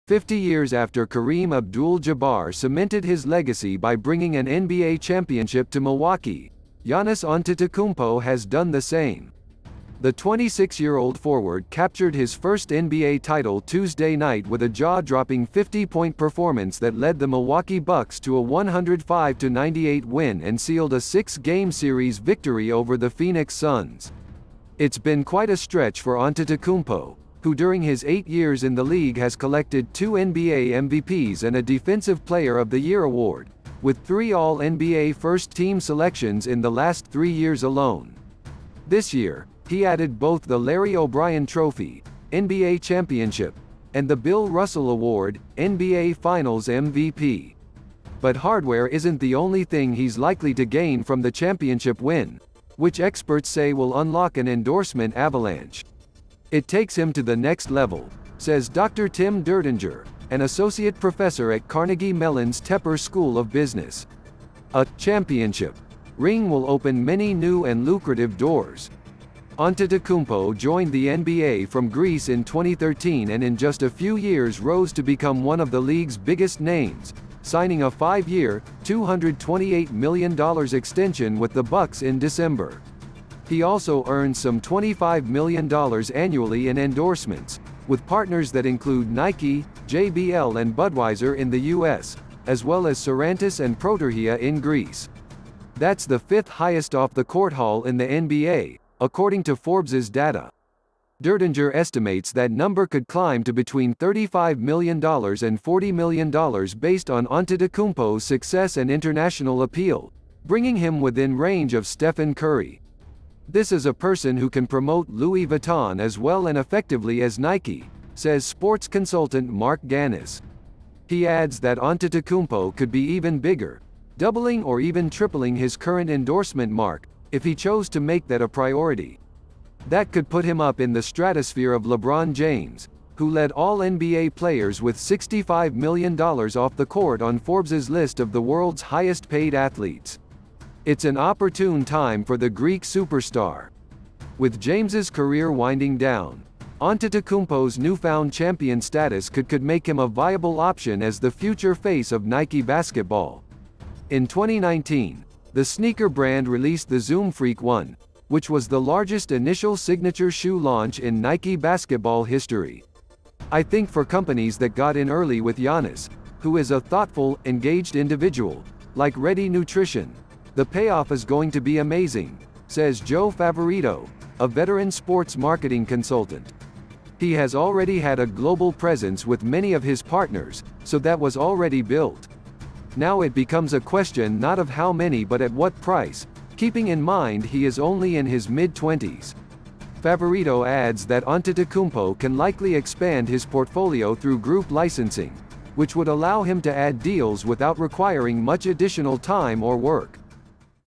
Now That Giannis Antetokounmpo Has An NBA Title Here’s How Much Money He
Fifty years after Kareem Abdul-Jabbar cemented his legacy by bringing an NBA championship to Milwaukee, Giannis Antetokounmpo has done the same. The 26-year-old forward captured his first NBA title Tuesday night with a jaw-dropping 50-point performance that led the Milwaukee Bucks to a 105-98 win and sealed a six-game series victory over the Phoenix Suns. It’s been quite a stretch for Antetokounmpo, who during his eight years in the league has collected two NBA MVPs and a Defensive Player of the Year Award, with three all-NBA first-team selections in the last three years alone. This year, he added both the Larry O’Brien Trophy (NBA championship) and the Bill Russell Award (NBA finals MVP). But hardware isn’t the only thing he’s likely to gain from the championship win, which experts say will unlock an endorsement avalanche.“It takes him to the next level,” says Dr. Tim Derdenger, an associate professor at Carnegie Mellon’s Tepper School of Business. “A [championship] ring will open many new and lucrative doors.”Antetokounmpo joined the NBA from Greece in 2013 and in just a few years rose to become one of the league’s biggest names, signing a five-year, $228 million extension with the Bucks in December. He also earns some $25 million annually in endorsements, with partners that include Nike, JBL and Budweiser in the U. S., as well as Sarantis and Protergia in Greece. That’s the fifth-highest off-the-court haul in the NBA, according to Forbes’ data. Derdenger estimates that number could climb to between $35 million and $40 million based on Antetokounmpo’s success and international appeal, bringing him within range of Stephen Curry.“This is a person who can promote Louis Vuitton as well and effectively as Nike,” says sports consultant Marc Ganis. He adds that Antetokounmpo could be even bigger, doubling or even tripling his current endorsement mark “if he chose to make that a priority.” That could put him up in the stratosphere of LeBron James, who led all NBA players with $65 million off the court on Forbes’ list of the world’s highest-paid athletes. It’s an opportune time for the Greek superstar. With James’ career winding down, Antetokounmpo’s newfound champion status could could make him a viable option as the future face of Nike basketball. In 2019, the sneaker brand released the Zoom Freak 1, which was the largest initial signature shoe launch in Nike basketball history.“I think for companies that got in early with Giannis, who is a thoughtful, engaged individual, like Ready Nutrition, the payoff is going to be amazing,” says Joe Favorito, a veteran sports marketing consultant. “He has already had a global presence with many of his partners, so that was already built. Now it becomes a question not of how many but at what price, keeping in mind he is only in his mid-20s.”Favorito adds that Antetokounmpo can likely expand his portfolio through group licensing, which would allow him to add deals without requiring much additional time or work.